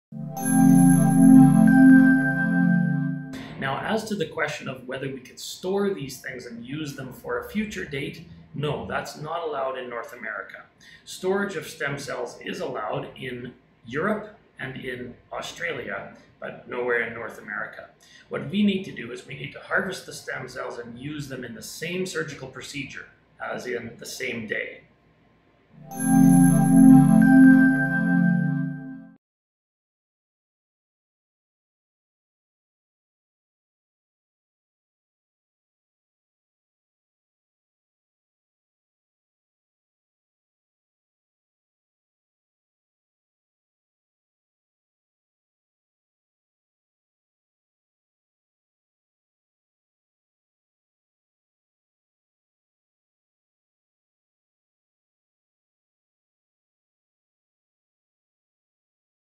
Are You Allowed to Store Stem Cells
Dr. Pagdin discusses whether or not your stem cells can be harvested and stored.